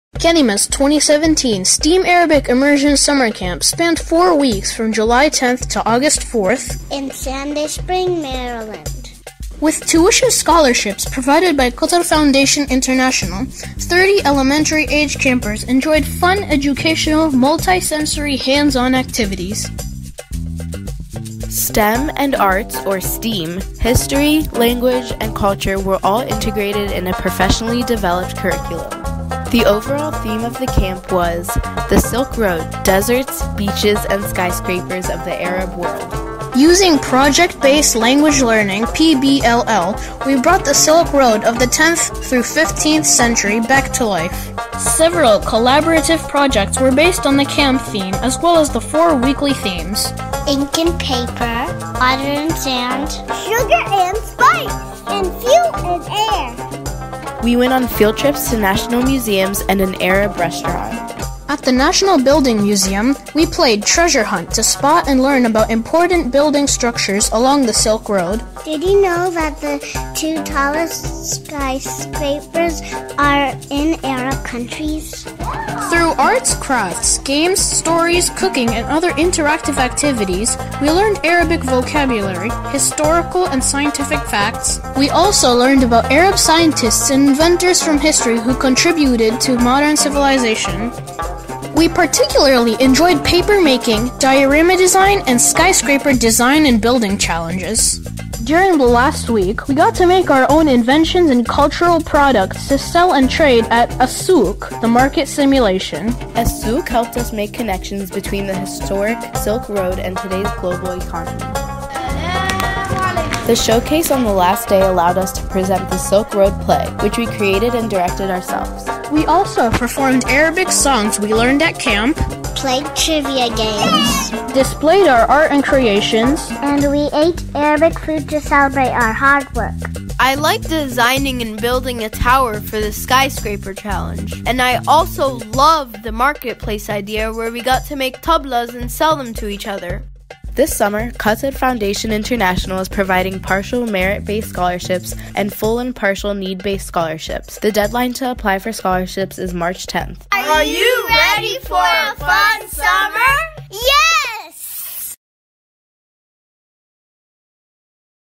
Kalimah's STEAM Arabic Immersion Summer Camps
Learn about Kalimah’s STEAM Arabic Immersion Summer Camps

2017 theme: “The Silk Road: Deserts, Beaches, and Skyscrapers of the Arab World”

2018 theme: "The Silk Road: Arab Lands as Trade Centers Past and Present"

For more info and to apply for 2018 summer camps scholarships, go and click "Arabic Immersion Programs". The deadline to apply has been extended to April 20.

Photo credits:
Qatar Foundation International
and
Kalimah Programs for Arabic Education